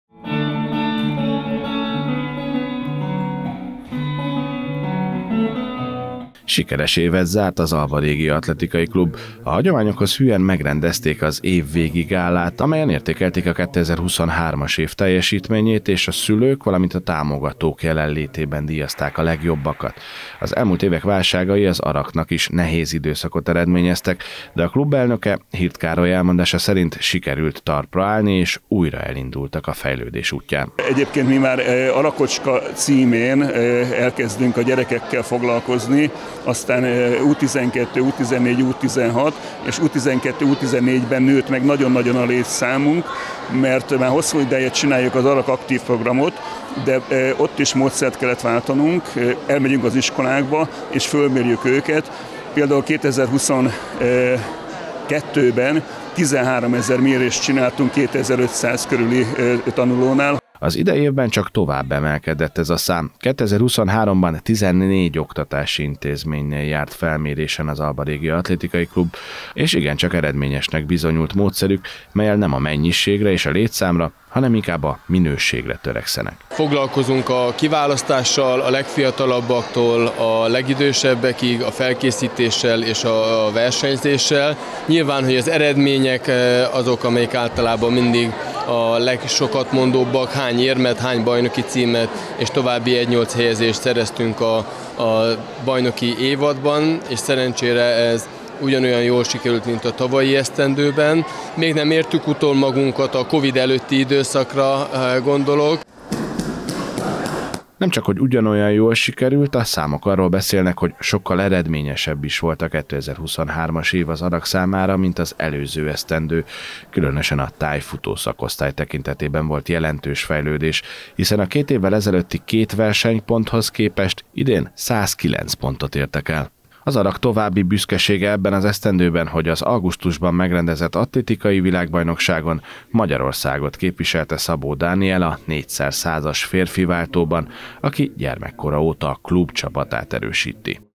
SIKERES ÉVET ZÁRT AZ ARAK
Évzáró gálát tartott az Alba Regia Atlétika Klub. Az ARAK sikeresnek könyvelte el a 2023-as évet, jelentős fejlődést és eredményeket értek el idén a klub sportolói.